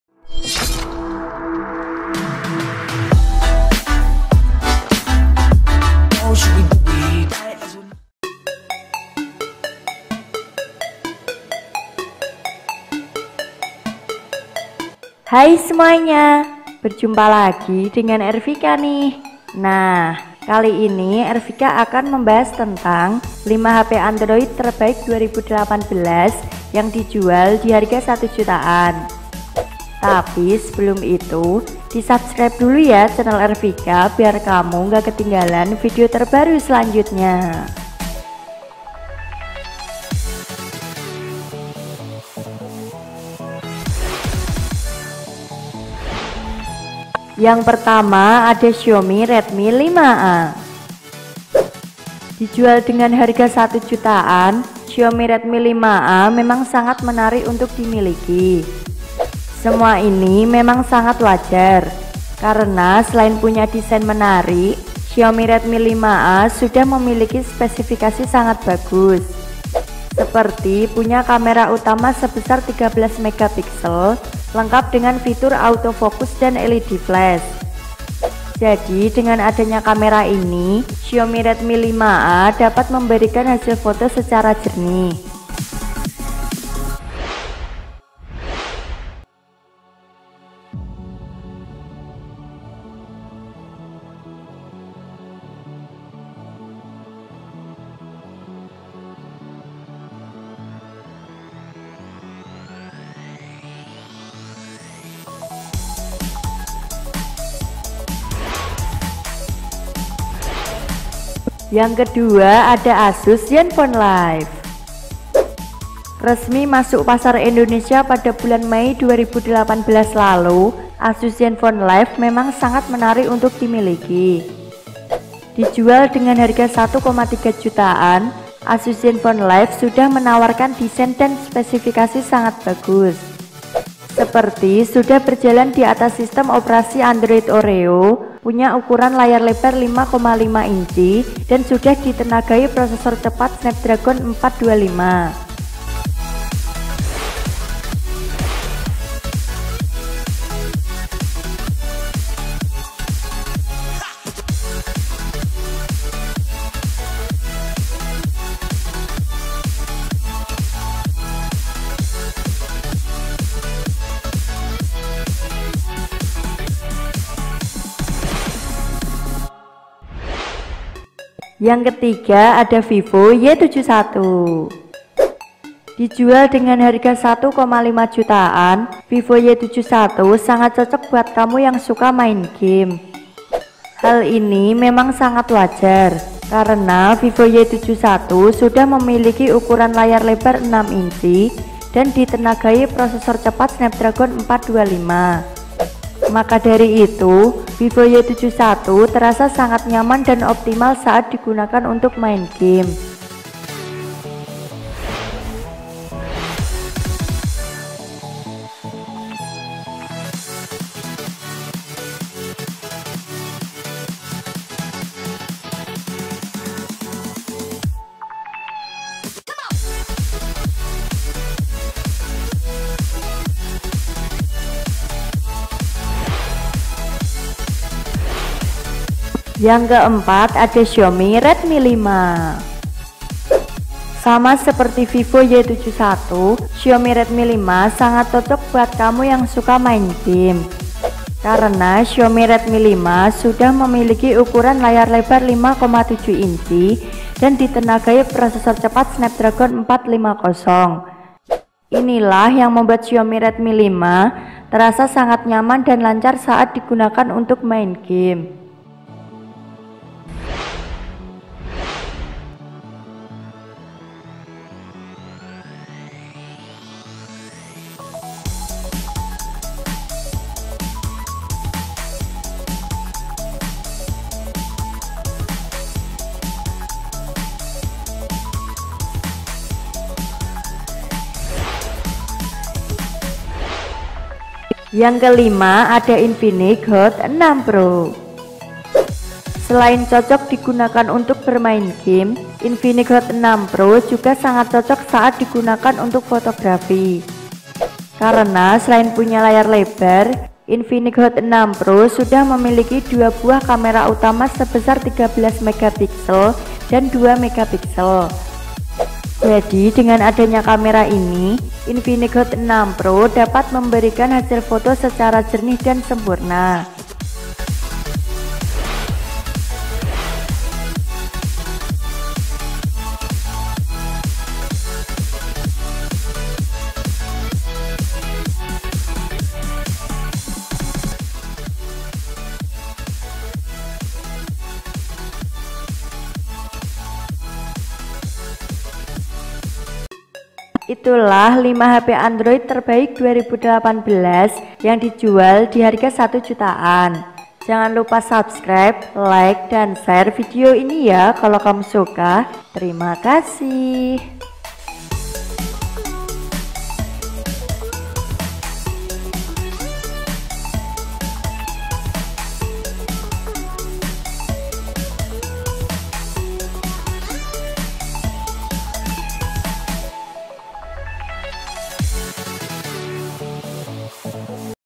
5 HP Android Harga 1 Jutaan Terbaik 2018
Hai semuanya berjumpa lagi dengan Ervika nih :)

Nah kali ini Ervika akan membahas tetang 5 HP Android Terbaik 2018 yang dijual di Harga 1 Jutaan.

Kira - kira hp apa sajakah itu ? Yuk langsung saja simak videonya sampai habis.

Daftar 5 HP Android Harga 1 Jutaan Terbaik 2018 :

1. Xiaomi Redmi 5A :  Rp 1 Jutaan
2. Asus Zenfone Live L1 : Rp 1,3 Jutaan
3. Vivo Y71 ( RAM 2 GB ) : Rp 1,5 Jutaan
4. Xiaomi Redmi 5 ( RAM 2 GB ) : Rp 1,5 Jutaan
5. Infinix Hot 6 Pro : Rp 1,5 Jutaan

Suber Harga : Lazada, Tokopedia dan Bukalapak

Kelima HP diatas saat ini sudah secara resmi masuk pasar Indonesia. Artinya sudah dapat kamu beli melalui toko online maupun offline diseluruh Indonesia.

NOTE : HARGA MUNGKIN BERBEDA, TERGANTUNG TEMPAT ATAU TOKO YANG MENJUALNYA.